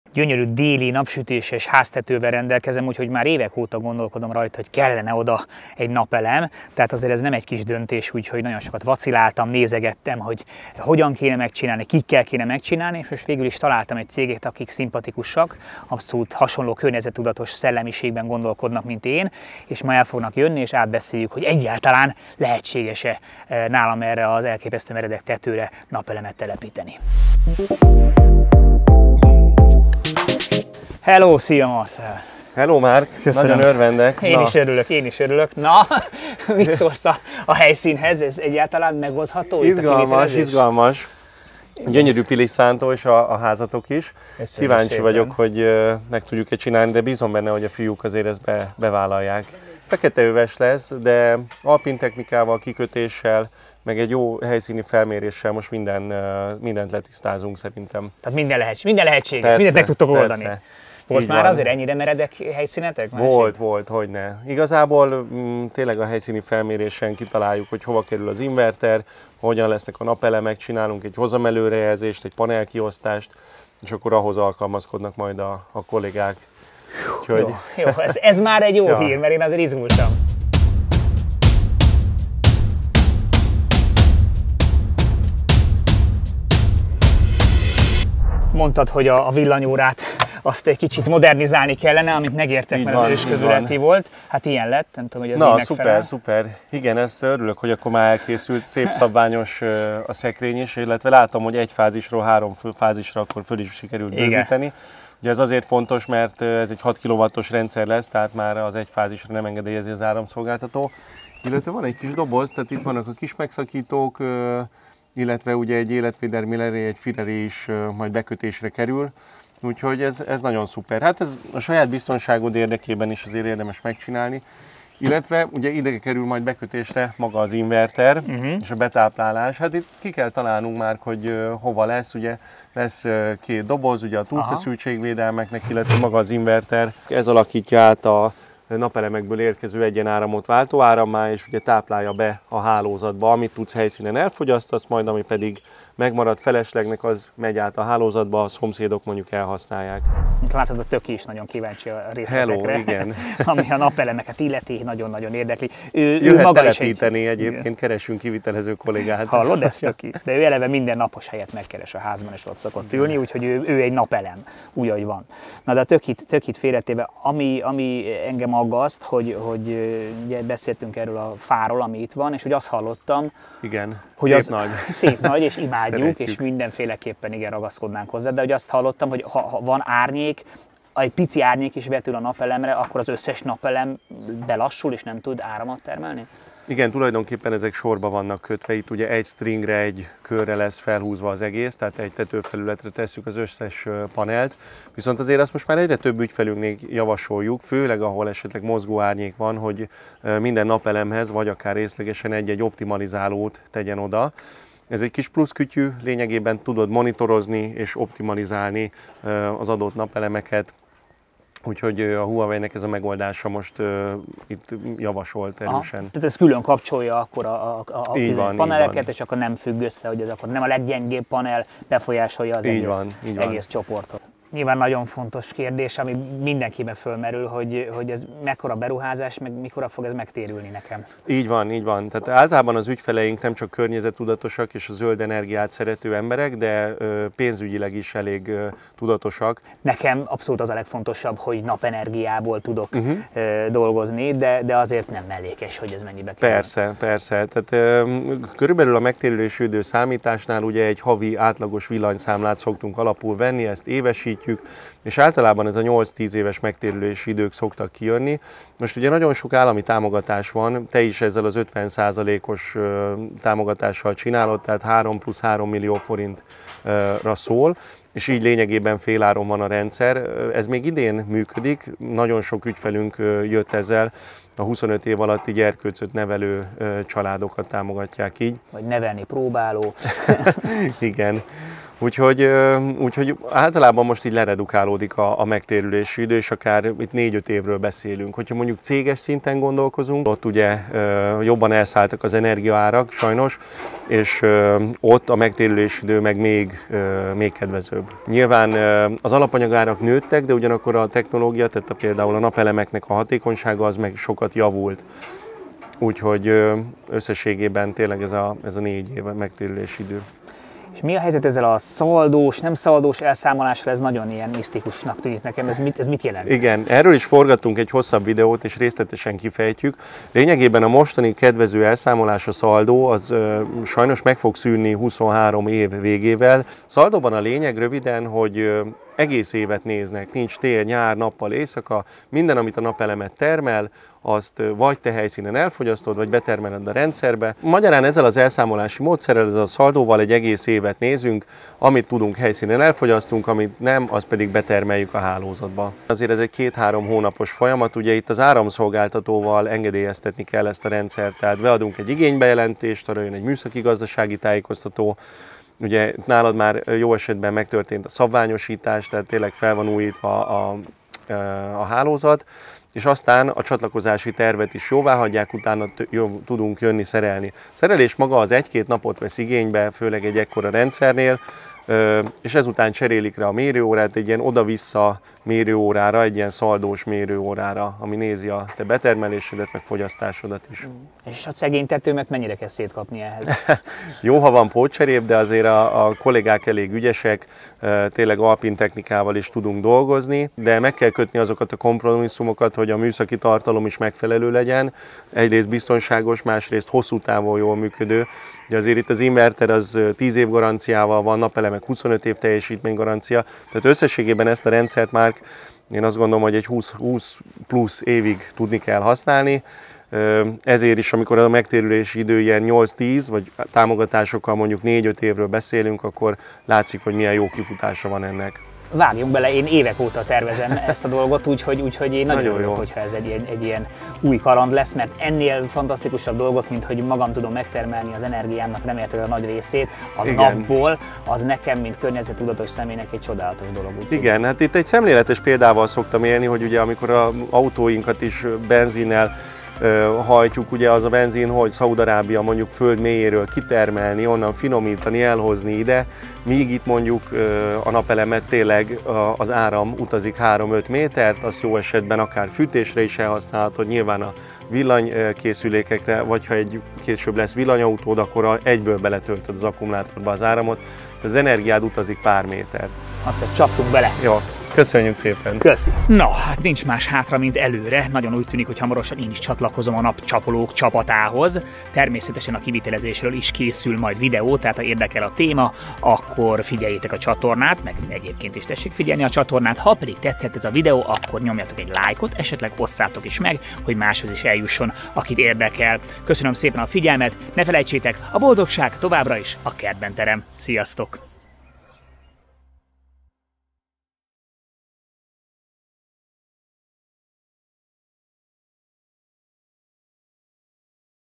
Megéri a napelem? | kert TV
Még néhány évvel ezelőtt is tudományos-fantasztikumnak, de legalábbis elérhetetlennek tűnt, hogy a hétköznapi emberek a nap ingyenes és tiszta energiáját felhasználva tudják ellátni otthonaikat árammal. Azt pedig csak a legmerészebb álmaimban reméltem, hogy majd egyszer én is egy ilyen "nap-szüretelőt" tudhatok a kis házikónk tetején.
Most ez az álom hajszálnyira van a megvalósulástól, már csak azt kell kideríteni, hogy a Solar-kit csapata képes lesz-e erre a meredek tetőre telepíteni a rendszert? Illetve, milyen egyéb előkészületekre van szükség,
És nem utolsó sorban, mikorra fog ez nekem megtérülni? Megéri Nekünk is a nepelem?

Olvass hasznos cikkeket a honlapunkon